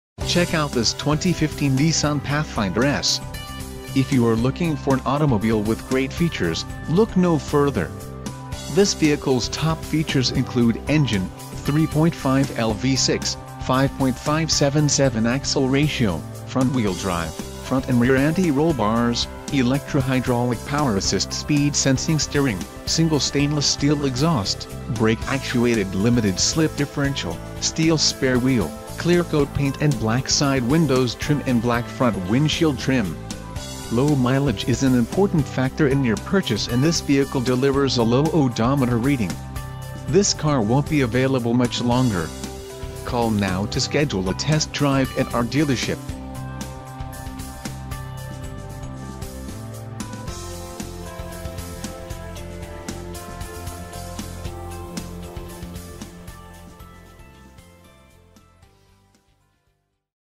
2015 Nissan Pathfinder S - Kelly Car - Emmaus, PA 18049
2015 Nissan Pathfinder S Condition: New Mileage: 7 Transmission: CVT Xtronic Exterior Color: Black Interior Color: Charcoal Doors: 4 Stock: N5T1014 Cylinders: 6